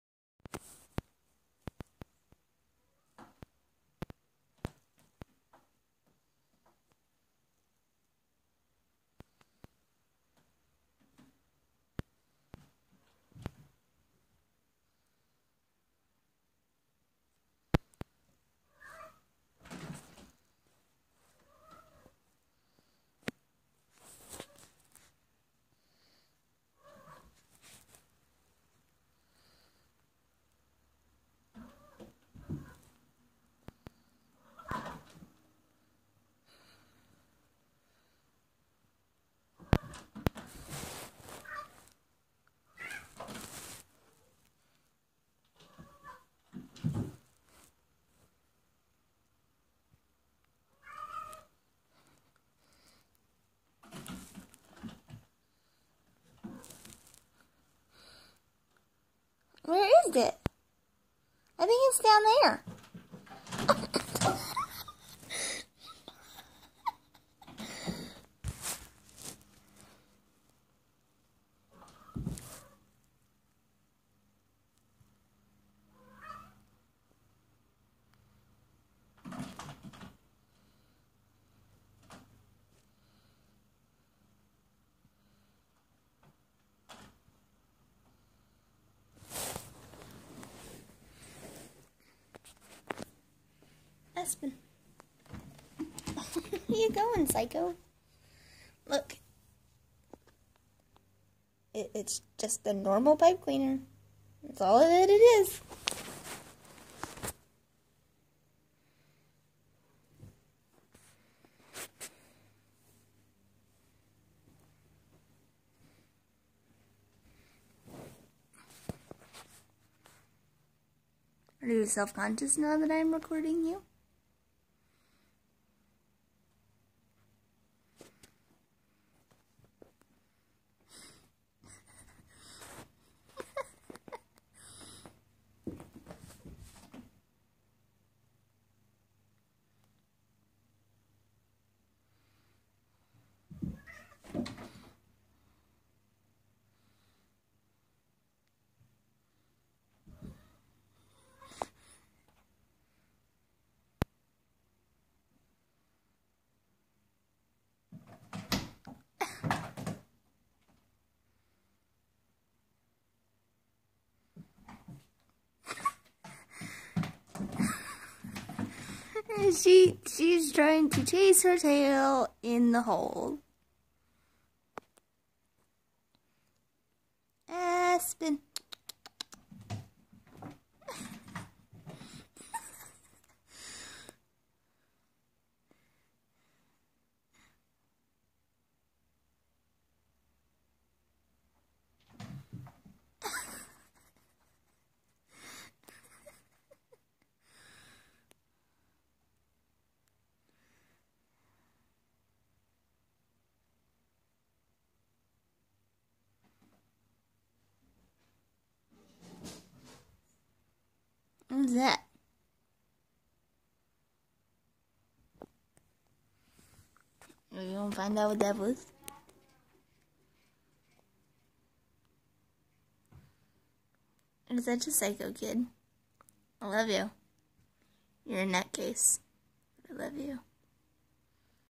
Black and white cat playing
This is Aspen, one of three crazy kitties. Sorry for the poor audio this is my first YouTube video and I don't have a decent recording device.